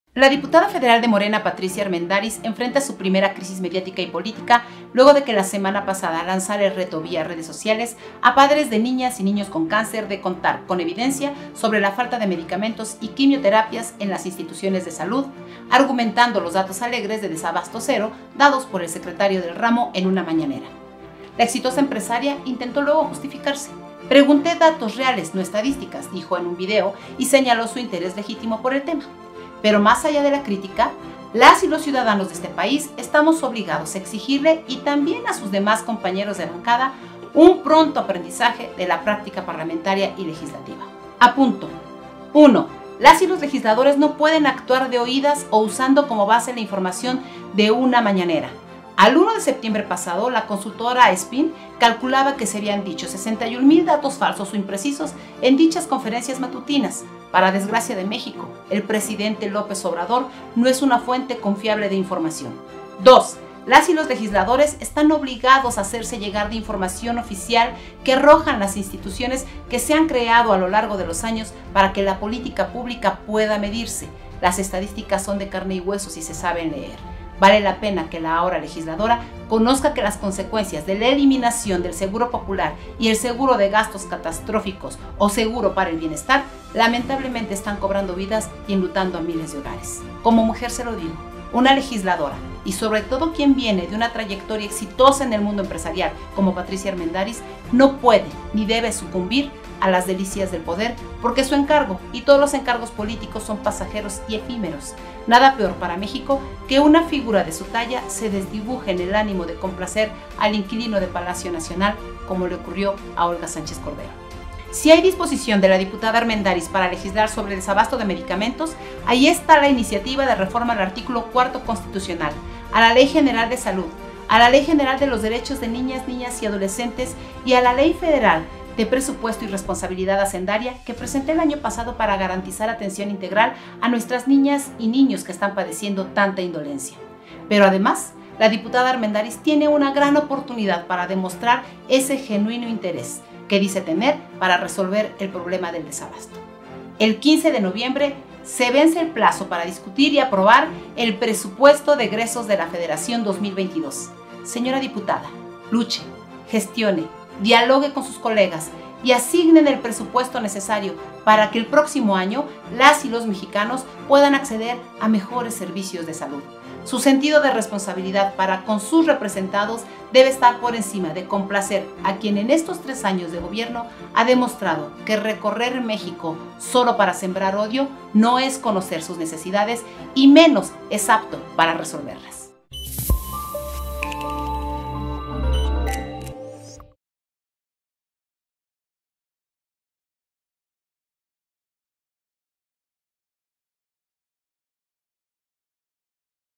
ADRIANA DÁVILA lanza RETO a la 4T y aclara que AMLO no es garantía de veracidad
"Las y los mexicanos merecemos mejor gobierno, mejor oposición y mayor justicia"
ADRIANA DÁVILA "La realidad en datos"
Si el interés de la diputada Armendariz para apoyar a los niños con cáncer es genuino -como dice-, debe convencer a sus compañeros de bancada, para que el Presupuesto de Egresos de la Federación 2022 considere  recursos suficientes para el sector salud.

Ahí está el reto.